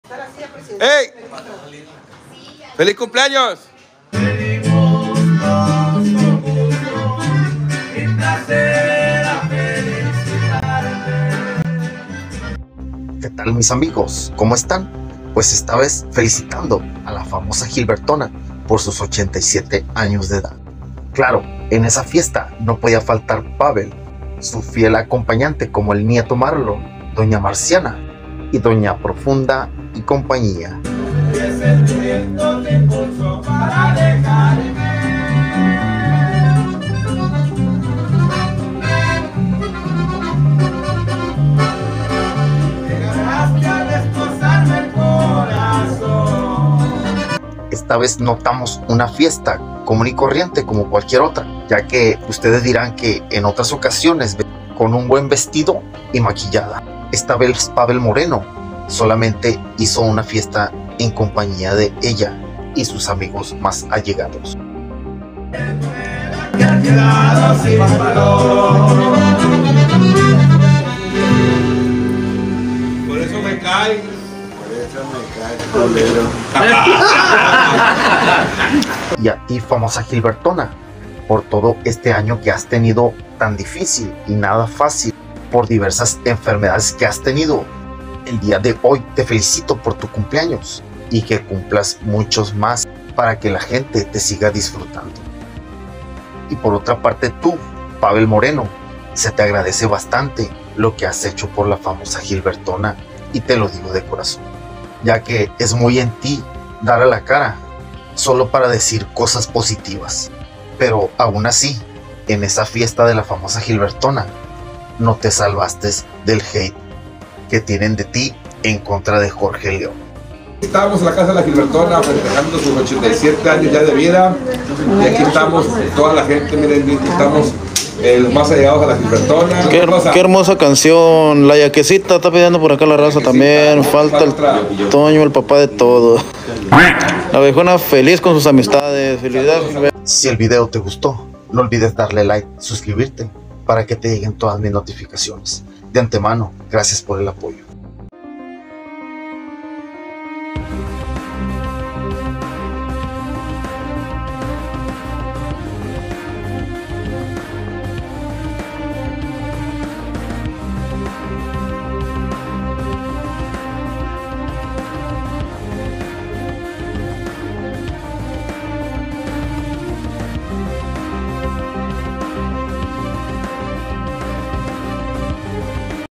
Cumpleaños "87" De La Famosa Gilbertòna🥳"Así le Festejaron El Día De Ayer🛑
el día de ayer cumplió años la famosa Gilbertòna y le festejaron sus 87 primaveras.

créditos para el canal.

sígueme por tiktok
juanrlive

sígueme por facebook.